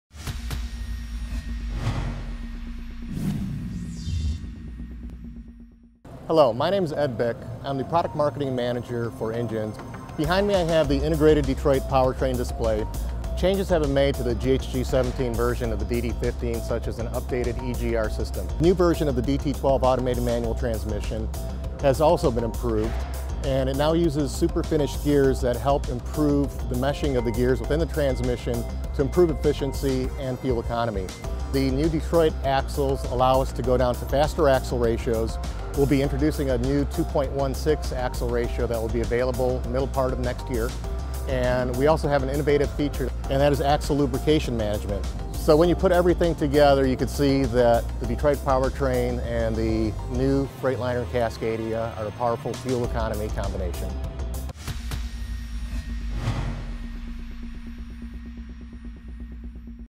The Integrated Detroit Powertrain on the newCascadia
Fuel efficiency is name of the Integrated Detroit Powertrain's game. See how it impacts the new Cascadia from MC&E Las Vegas.